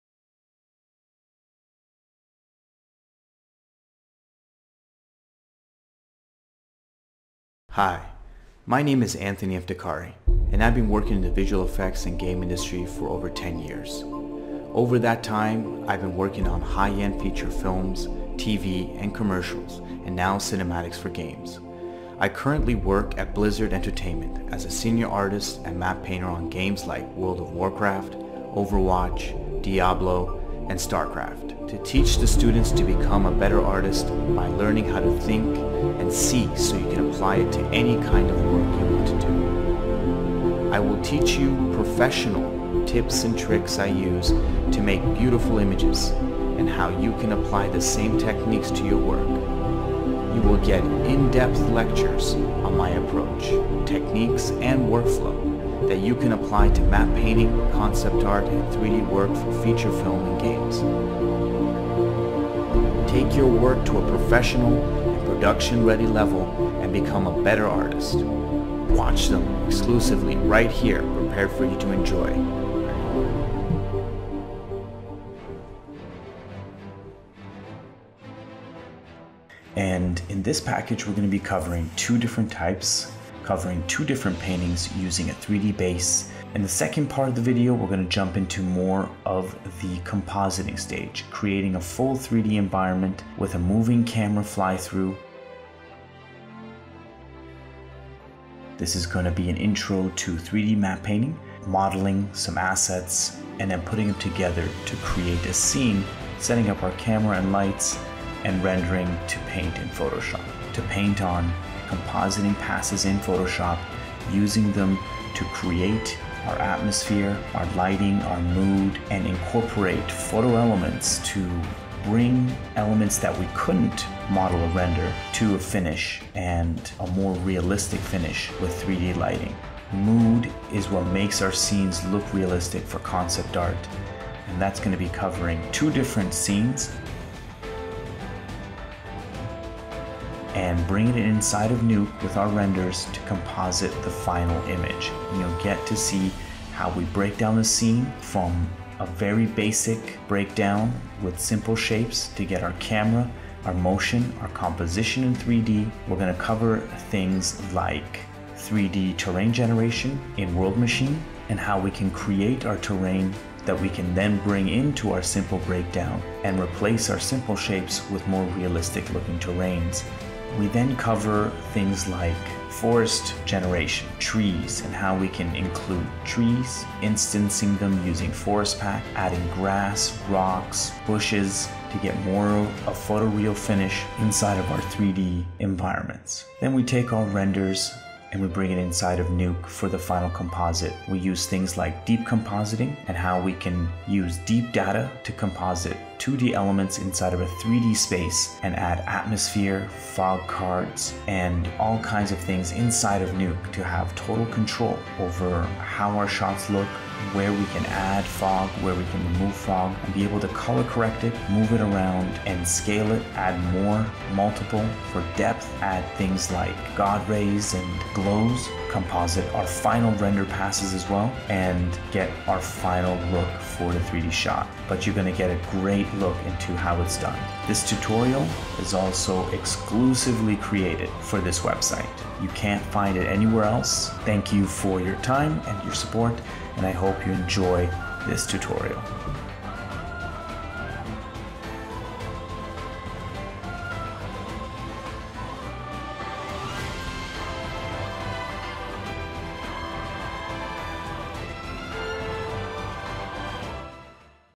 Trailer for The Latest Exclusive Tutorial 3D CG Cinematic Scene Design
Do you want to explore the mystery of CG Legend Blizzard?Anthony Eftekhari will tell you in this exclusive tutorial of 3D Concept Composition and CG Cinematic Scene Design.
It contains three seperated cases with explanation of 3D aided concept design and dynamic scenes composition workflow on Nuke.